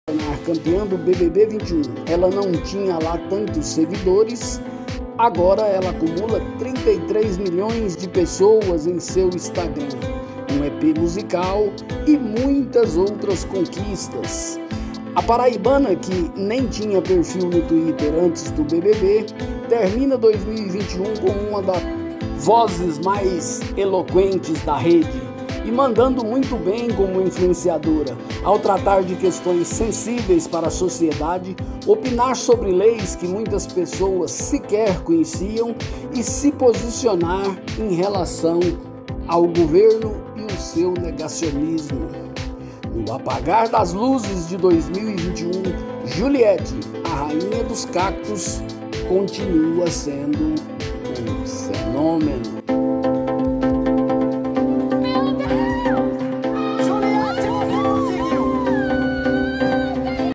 Quem vê Juliette dominando as redes sociais nem imagina que ela tinha "vergonha" de gravar stories
Que mudança de postura e como aprende rápido. Juliette tinha vergonha de gravar vídeos para seus stories, não se sentia à vontade quando anônima.

Motivos não faltaram para tanta celebração no ano de grande virada de sua vida. Se antes de tornar campeã do "BBB 21", ela não tinha lá tantos seguidores, agora ela acumula 33 milhões de pessoas em seu Instagram, um EP musical e muitas outras conquistas.

A Paraibana que nem tinha perfil no Tweter antes do BBB, termina 2021 como uma das vozes mais influentes da rede, e mandando muito bem como influenciadora ao tratar de questões sensíveis para sociedade, opinar sobre leis que muitas pessoas sequer conheciam e se posicionar em relação ao governo e o negacionismo.

No apagar das luzes de 2021, Juliette, a Rainha dos cactos, listou os 10 momentos que marcaram sua trajetória no ano